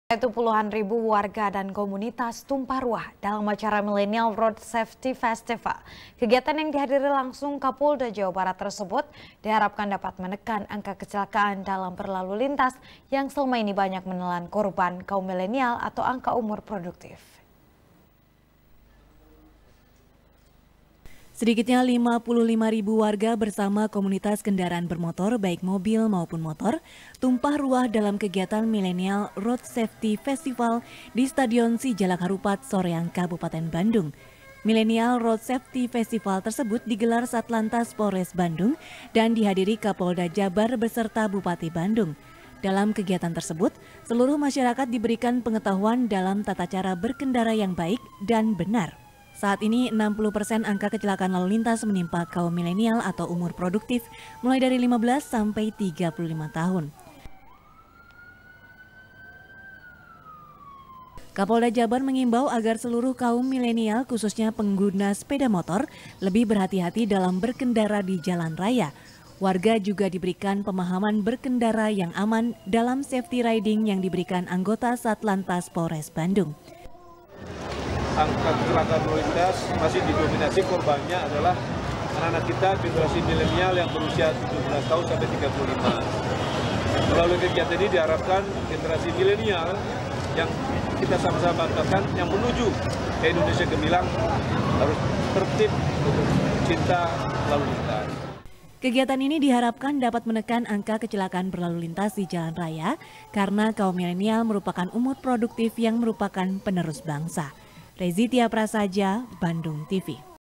55.000 Warga Rayakan Acara Milenial Road Safety | SBR | BANDUNGTV
SBR | BANDUNGTV | NIKEN PRATIWI | Puluhan ribu warga dan komunitas tumpah ruah dalam acara milenial road safety festival. kegiatan yang dihadiri langsung kapolda jawa barat tersebut, diharapkan dapat menekan angka kecelakaan dalam berlalu lintas, yang selama ini banyak menelan korban kaum milenial atau angka umur produktif.